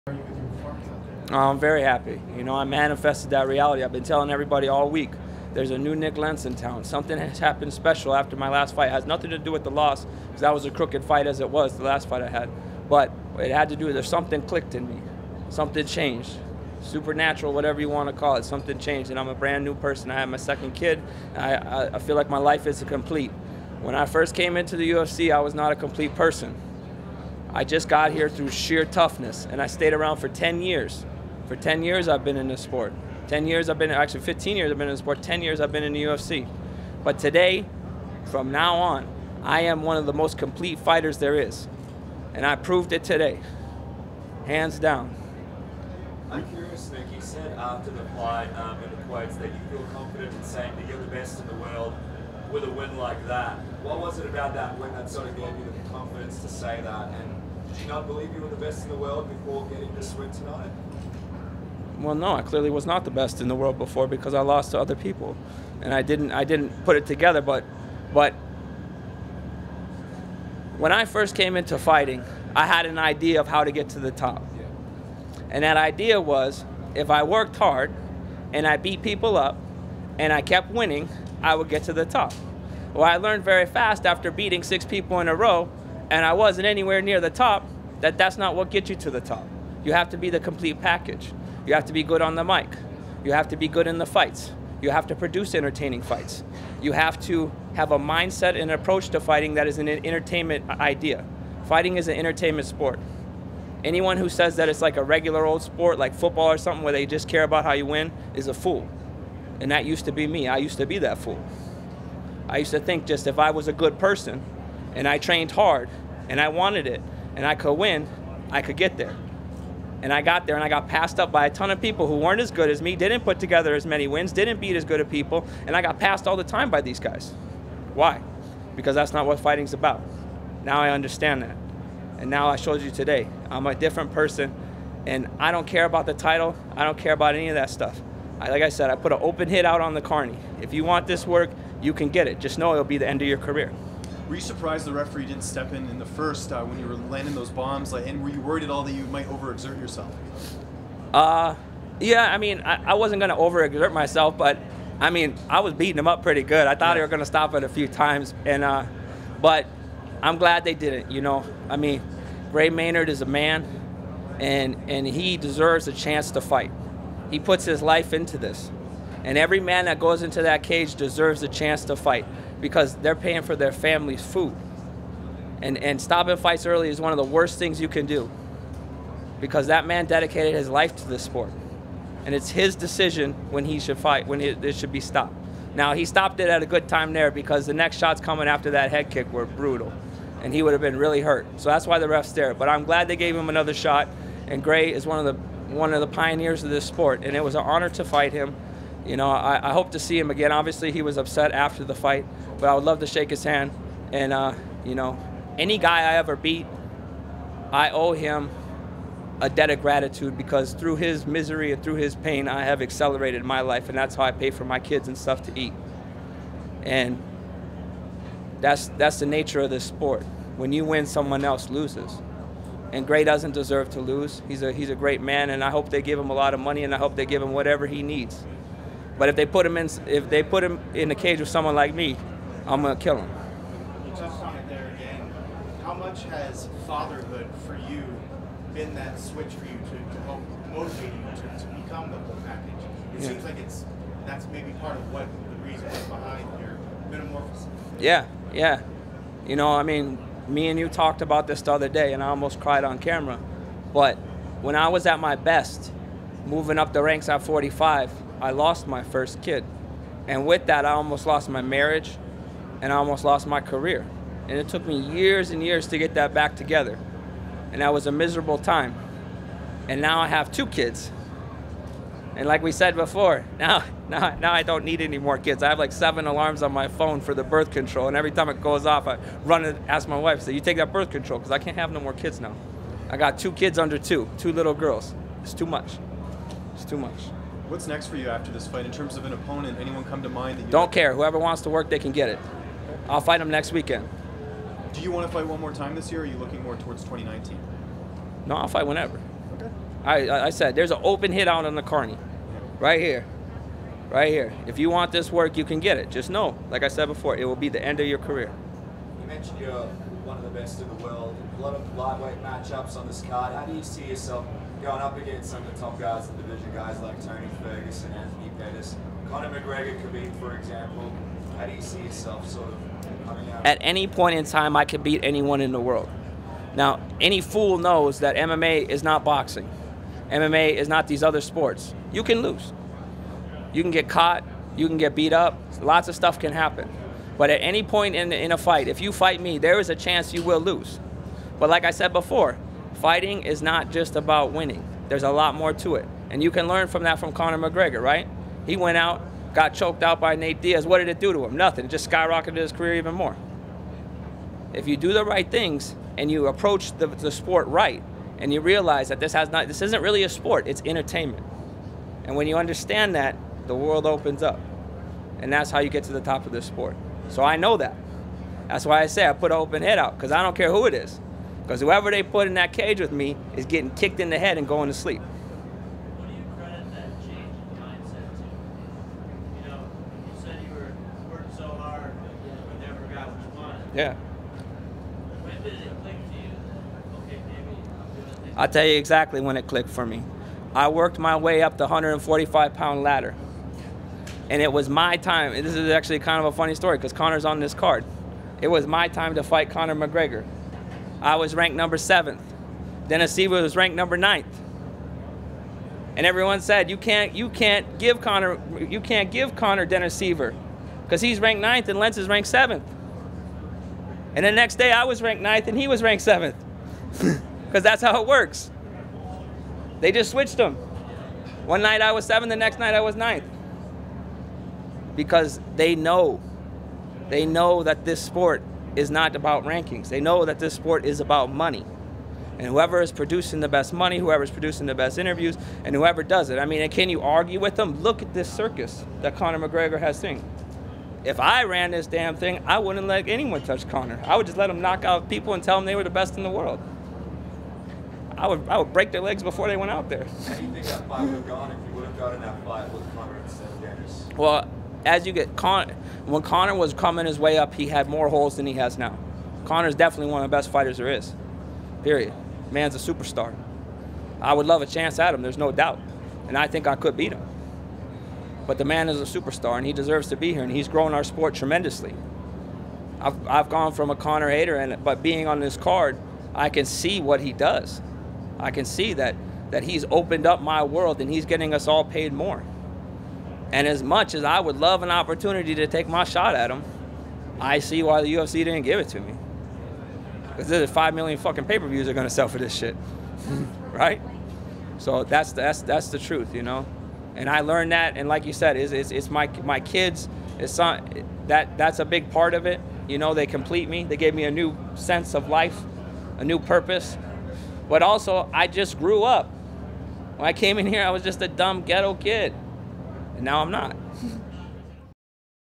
UFC 229: Nik Lentz Explains How He Went From Hating To Respecting Conor McGregor - MMA Fighting
UFC lightweight Nik Lentz explains how having kids has changed his outlook on life, his admiration for Gray Maynard, the open hit he put out on himself and how his perspective of Conor McGregor has changed over the years.